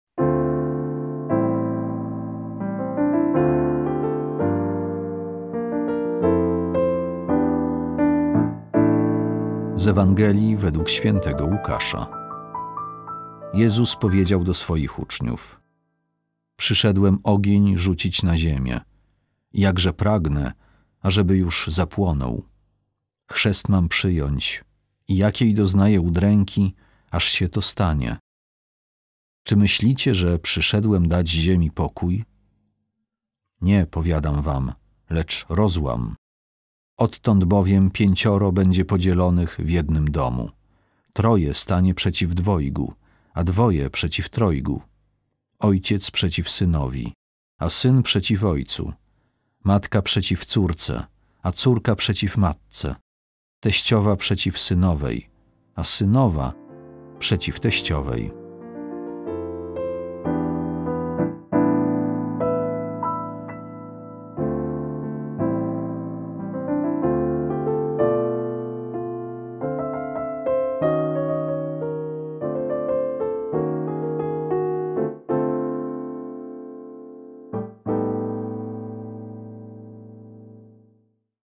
Ewangelia | 22 października 2020
22 października 2020
Jezus powiedział do swoich uczniów:
«Przyszedłem ogień rzucić na ziemię i jakże pragnę, ażeby już zapłonął. Chrzest mam przyjąć, i jakiej doznaję udręki, aż się to stanie.